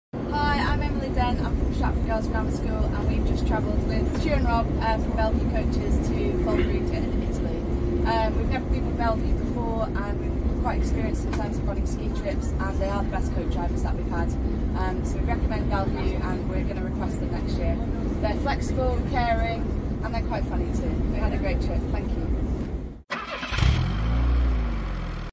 From Stretford to Italy! 🎿 🇮🇹 Belle Vue Coaches testimonial Stretford Grammar Girls School 🚌 ✨
From Stretford to Italy! 🎿 🇮🇹  We're excited to share a lovely testimonial from our teacher Emily at Stretford Girls Grammar School following their school skiing trip to Italy.

A huge shoutout to our drivers Stu and Rob for providing a fantastic and enjoyable travel experience! 🚌 ✨ 🏆

We're glad you all had a great time! 🕺